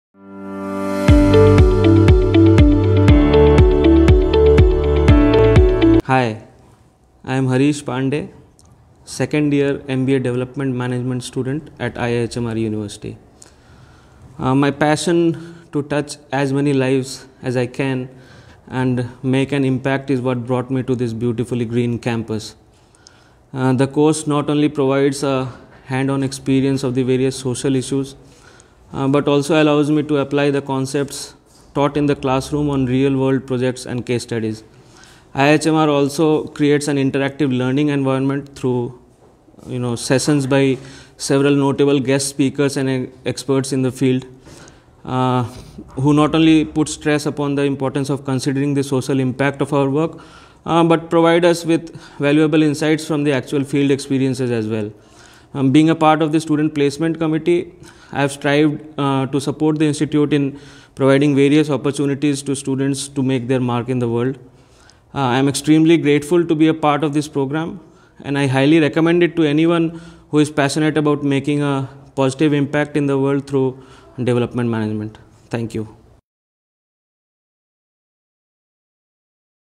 Harish Pandey's Journey at IIHMR University, Jaipur
Know about Harish Pandey's stint at IIHMR University.

As an MBA Development Management student, Harish shares how the university became a stepping stone in his personal and professional growth. He speaks about his passion for making a difference in the healthcare sector and how the resources and opportunities available at the university equipped him with the necessary skills and knowledge to excel in his field.

Listen to his inspiring story and do share your thoughts. Watch out for more student success stories!